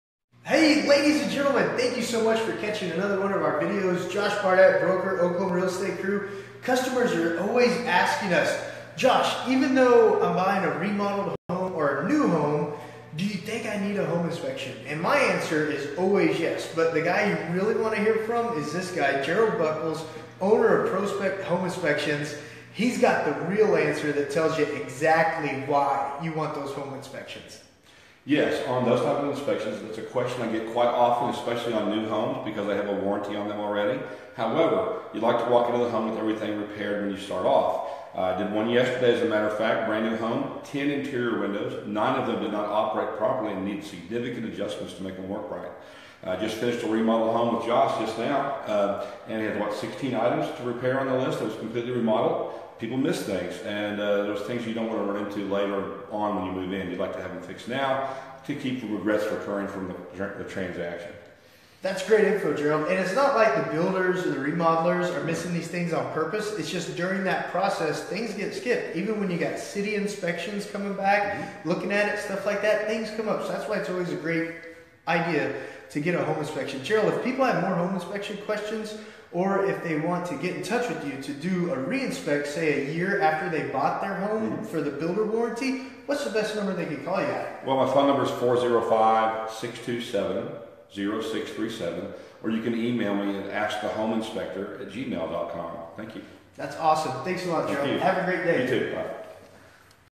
🏘️ Your Real Estate FYI 🏘️ Should I Have a New Home Inspected? 🏘️
Should I get a Home Inspection on New Construction or Remodeled Home? | Josh Barnett, Broker

Buying a New home or a remodeled Home?

Wondering if you need a Home Inspection?

Check out this video, it will give you more information to make a decision.

Video of informational purposes ONLY, you make your own decision.

Josh Barnett, Broker with eXp Realty